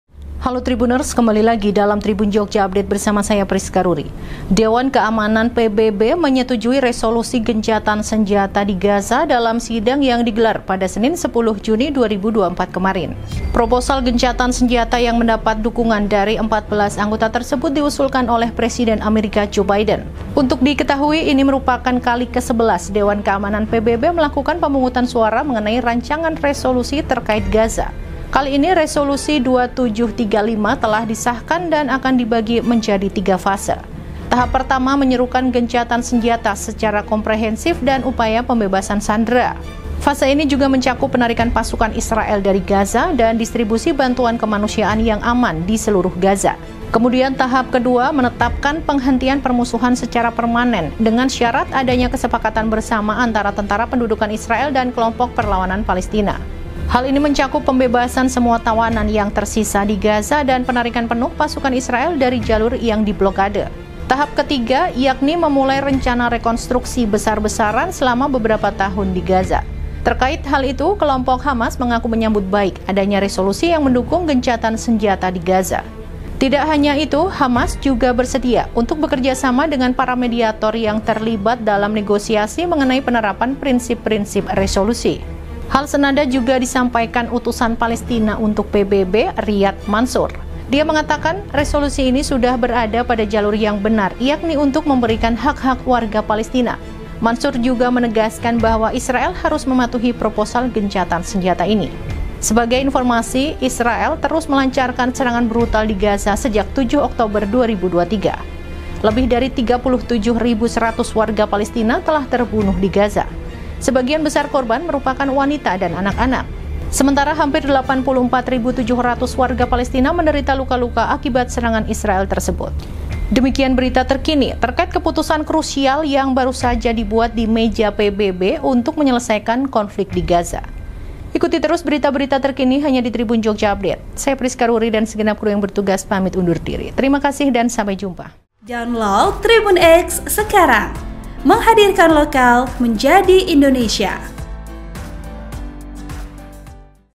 KEPUTUSAN KRUSIAL BARU SAJA DIBUAT DI MEJA PBB, AKANKAH KONFLIK DI GAZA BERAKHIR?
Dewan Keamanan PBB menyetujui resolusi gencatan senjata di Gaza dalam sidang yang digelar pada hari Senin, 10 Juni 2024 kemarin.

Proposal gencatan senjata yang mendapat dukungan dari 14 anggota tersebut, diusulkan Presiden Amerika, Joe Bidden.

Untuk diketahui, ini merupakan kali kesebelas DK PBB melakukan pemungutan suara mengenai rancangan resolusi terkait Gaza.

Editor Video : Veri Vesiano
Narator : Prisca Ruri
Editor Naskah : Mona Kriesdinar
Produser : Ribut Raharjo
(vsn)